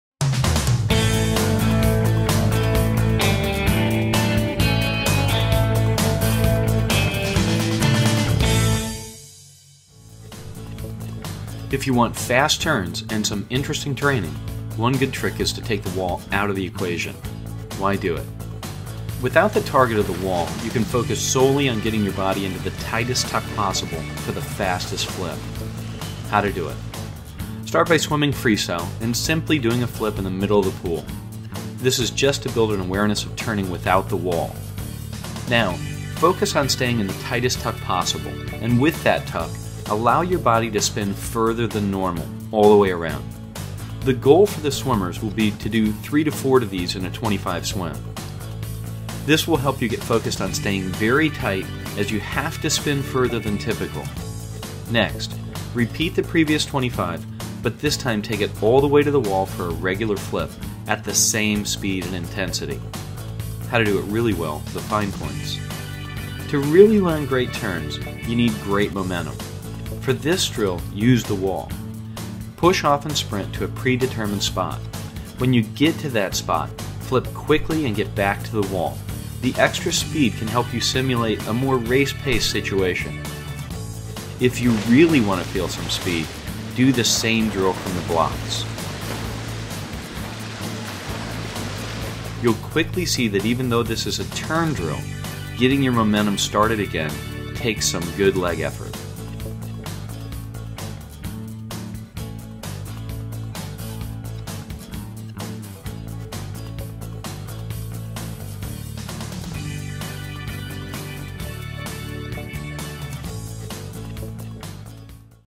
Turns - Mid-Pool Flips
Turns - Mid-Pool Flips from GoSwim.tv. Like this? Watch the latest episode of GoSwim.tv on Blip!

If you want FAST turns and some interesting training, one good trick is to take the wall out of the equation.

Why do it:
Without the target of the wall, you can focus solely on getting your body into the tighest tuck possible, for the fastest flip.

How to do it:
1)  Start by swimming freestyle, and simply doing a flip in the middle of the pool.  This is just to build an awareness of turning without the wall.
2)  Now, focus on staying in the tighest tuck possible, and with that tuck, allow your body to spin further than normal, all the way around.  The goal for the swimmers will be to do 3-4 of these turns in a 25 swim.  This will help you get focused on staying very tight as you have to spin further than typical.
3)  Next, repeat the previous 25, but this time take it all the way to the wall for a regular flip... at the same speed and intensity.

How to do it really well (the fine points):
To really learn great turns, you need some great momentum.  For this drill, use the wall.  Push off and sprint to a pre-determined spot.  When you get to that spot, flip quickly and get back to the wall.  The extra  speed can help simulate a more race pace situation.

If you REALLY want to feel some speed, do this same dril from the blocks.  Youapos;ll quickly see that even though this is a turn drill, getting your momentum started again takes some good leg effort.